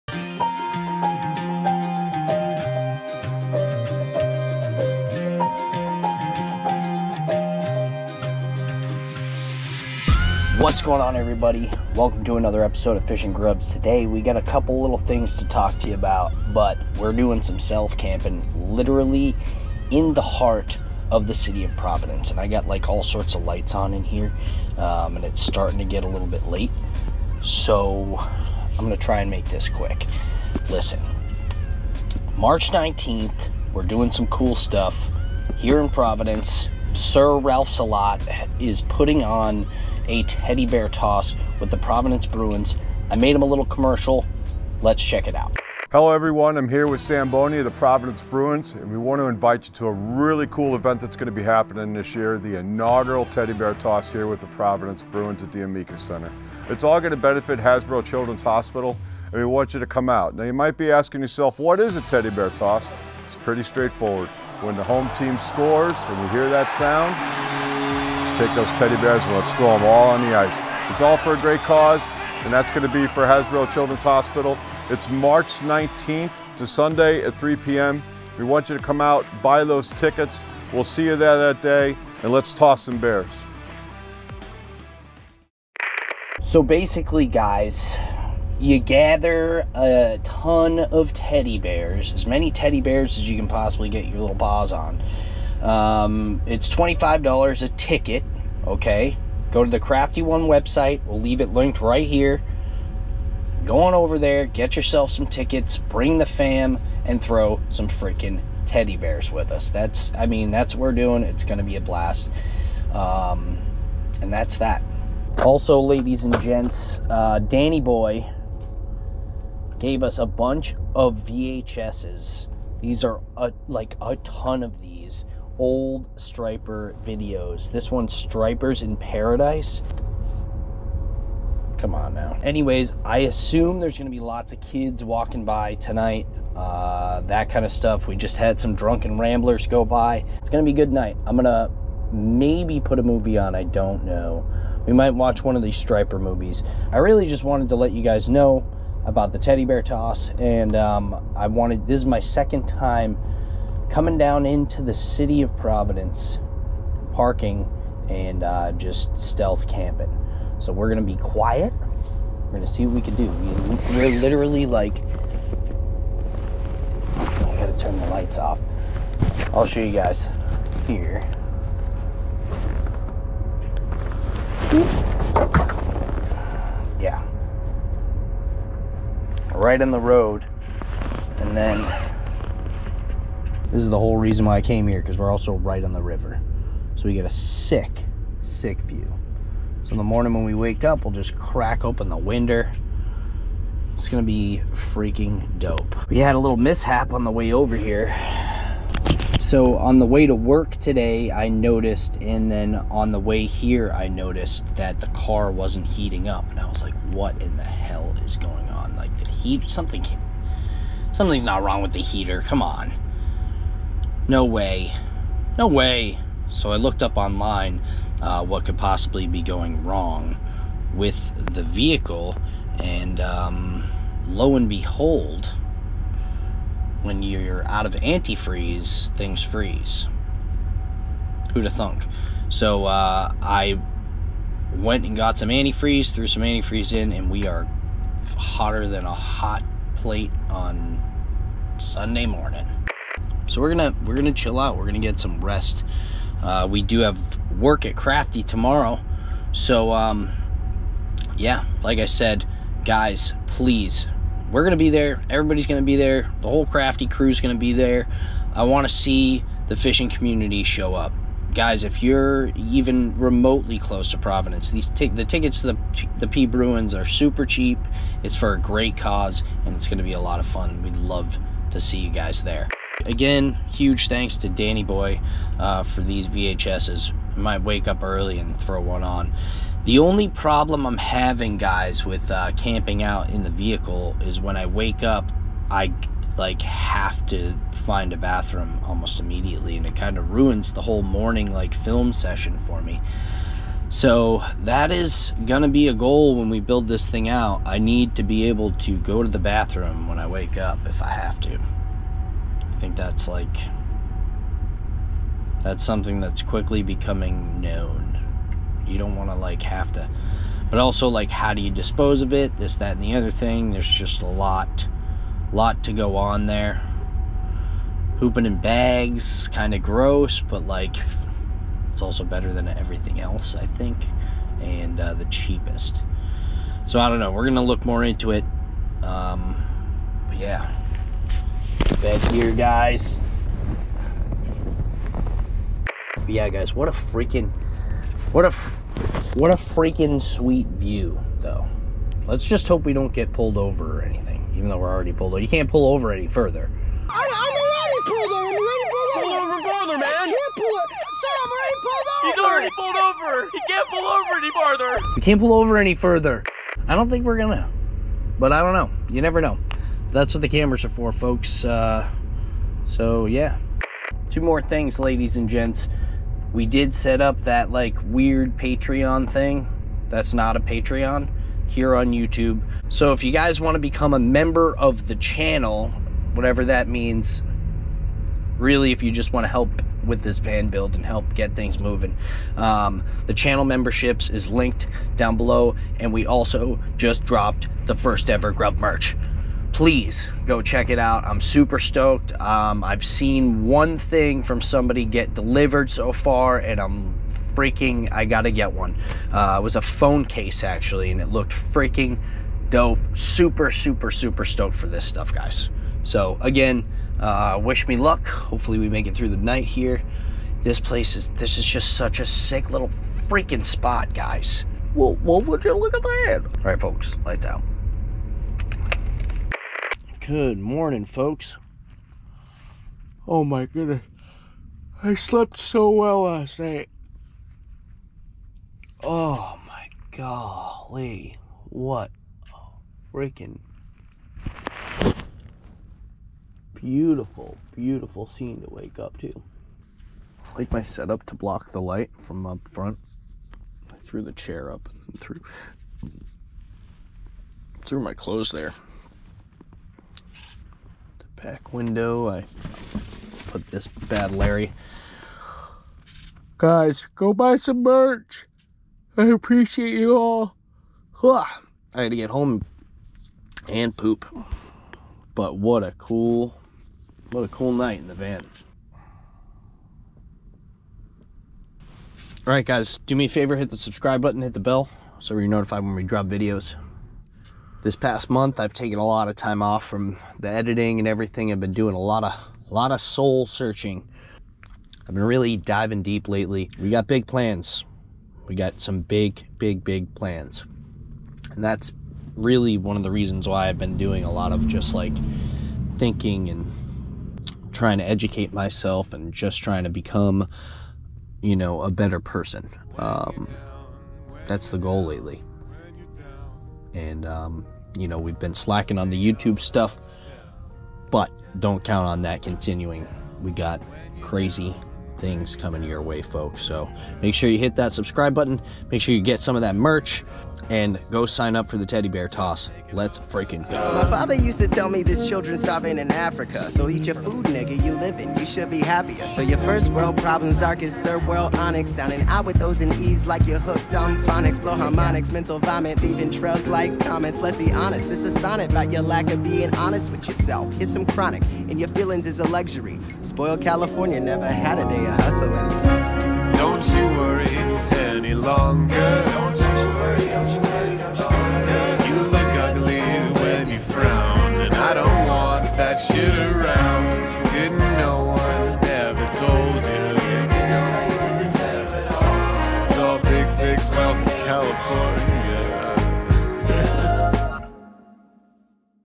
Stealth Van Camping in the CITY! Providence Bruins Teddy Bear Toss Announcement!
Doing some stealth van camping in the city and talking some updates!